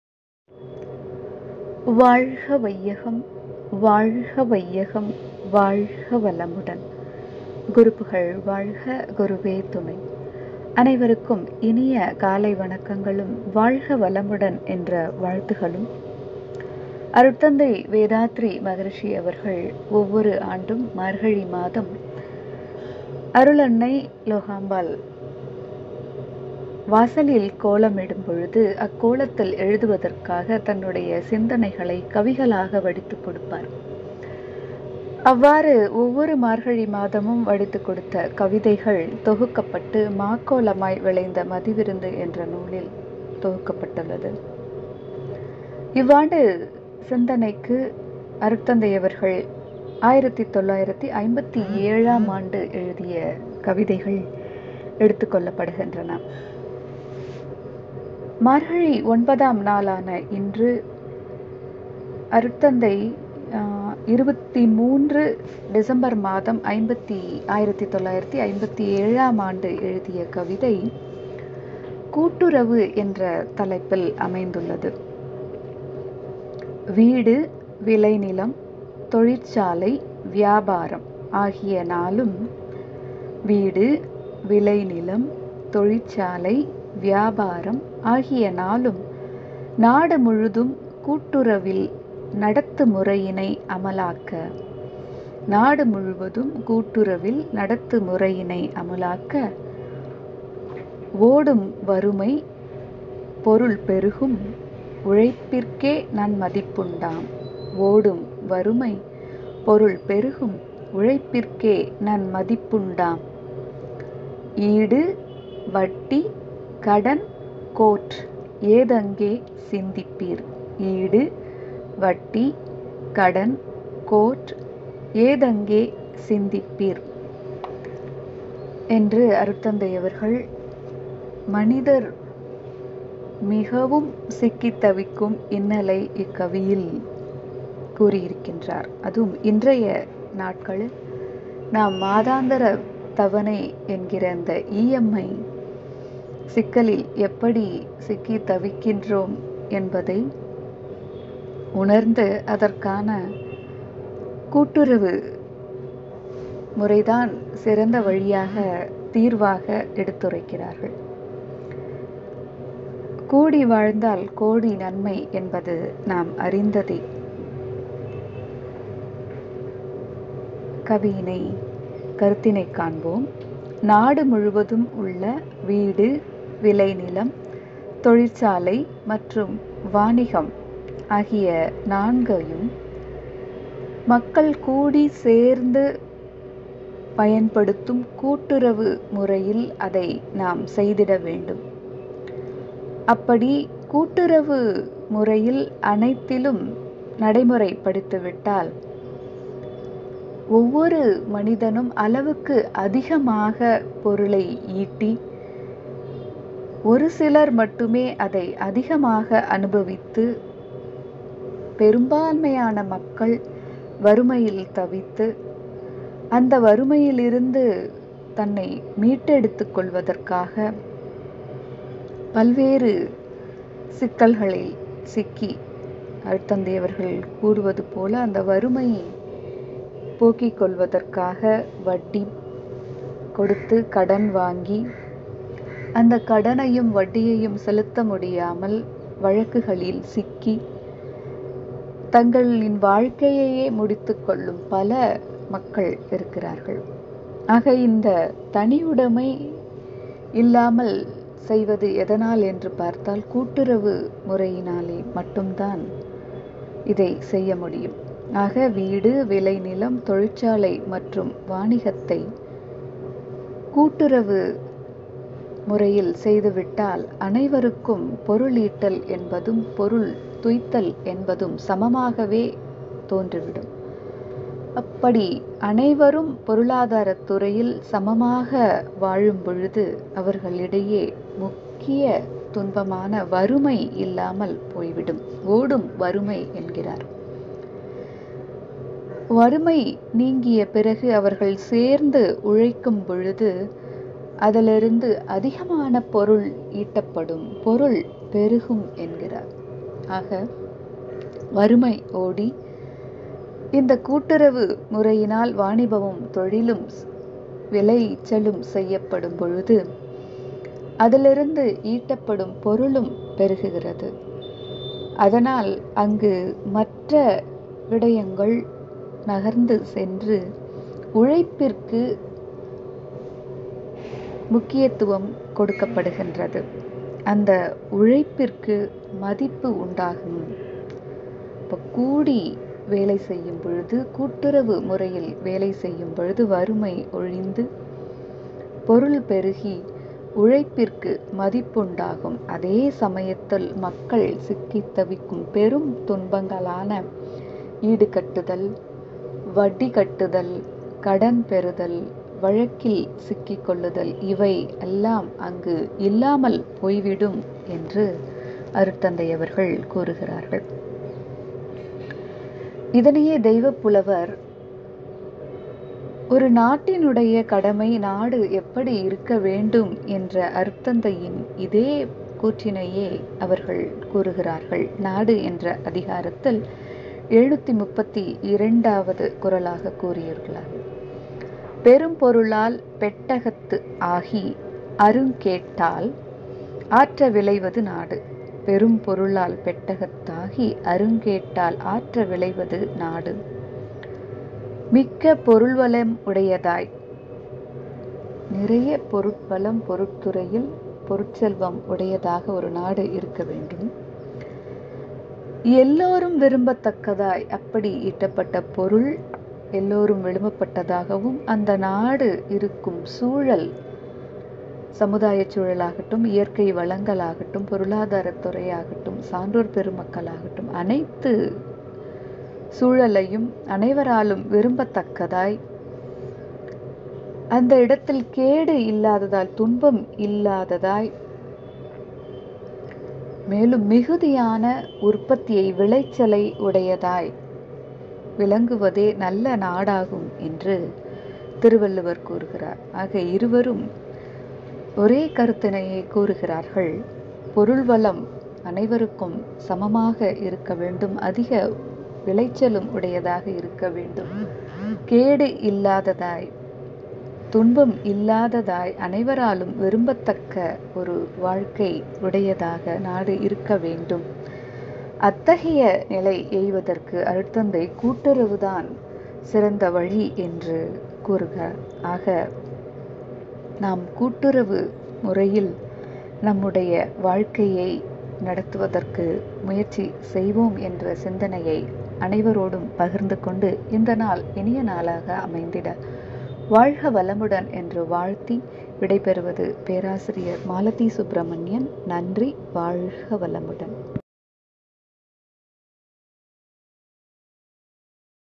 மாக்கோலமாய் விளைந்த மதி விருந்து - கூட்டுறவு || திருக்குறளும் வேதாத்திரியமும்
வாழ்க வளமுடன்

மாக்கோலமாய் விளைந்த மதிவிருந்து 2022

அன்னை லோகாம்பாள்  ஒவ்வொரு ஆண்டும் மார்கழி மாதத்தில் அதிகாலையில் கோலமிட்டு அதில் அருட்தந்தை கூறும் கவிகளை எழுதுவது வழக்கம். அவ்வாறு எழுதிய கவிகள் தொகுக்கப்பட்டு மாக்கோலமாய் விளைந்த மதிவிருந்து என்ற நூலாக தொகுக்கப்பட்டுள்ளது.  இப்பதிவுகள் அக்கவிகளுக்கு  விளக்கம் தருபவையாகும்.

இவ்வாண்டு வேதாத்திரி மகரிசியின் (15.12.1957 மார்கழி ஒன்று முதல் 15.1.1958 தை ஒன்று வரை பாடிய பாடல்களுக்கு பொருத்தமான திருக்குறளும் மனவளக்கலை பேராசிரியர் ச. மாலதி  சுப்பிரமணியன் அவர்களால் வழங்கப்படுகிறது..